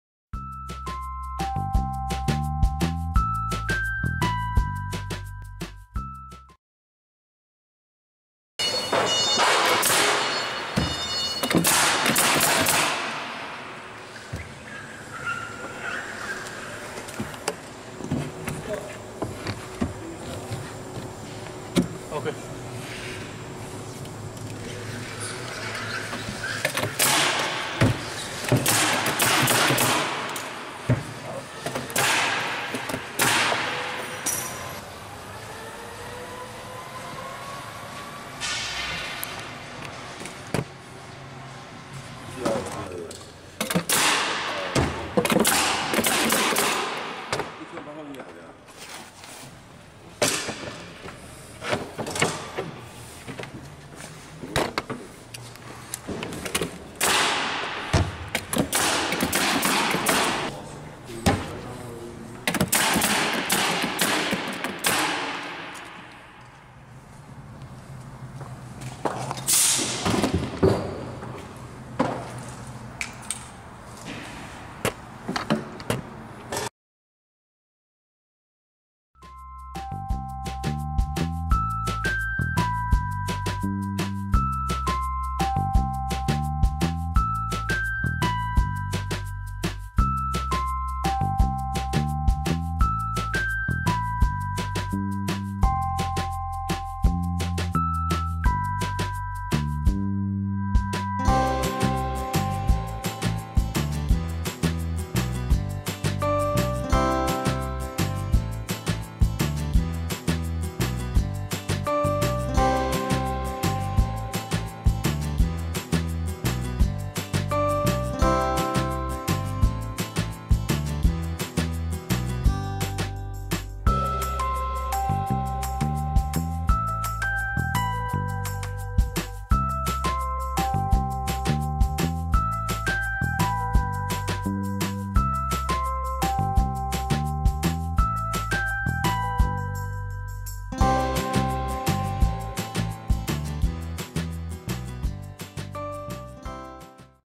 Cabinet Assembly Tool: Spotnail X1C1008
Specially made cabinet assembly tool for cabinets with face frames.
Active Sales Co., Inc.
Please call for more info.